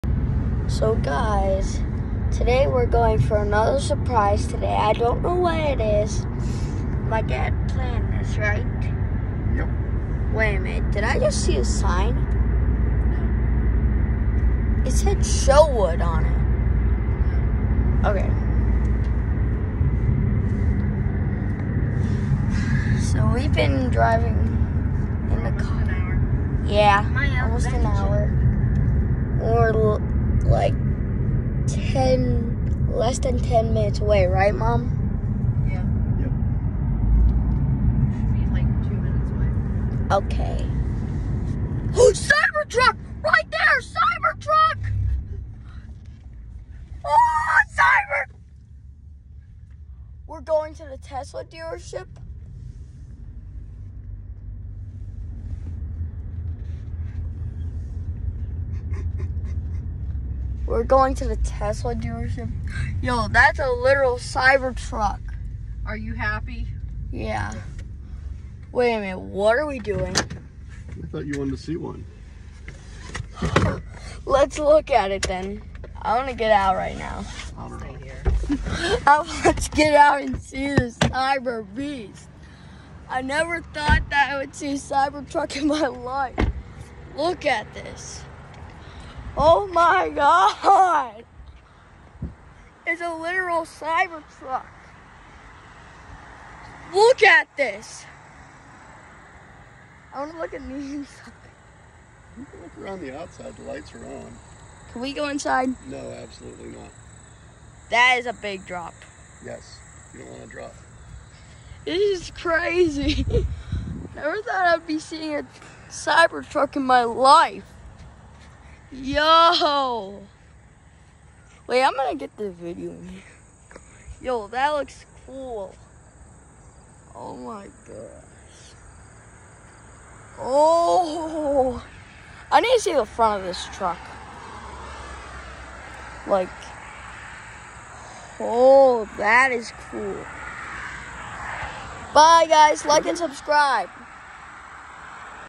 Today’s secret excursion to see the Tesla Cybertruck!
I finally got to see and touch a cyber truck ! Cool truck Elon ! Can I drive it?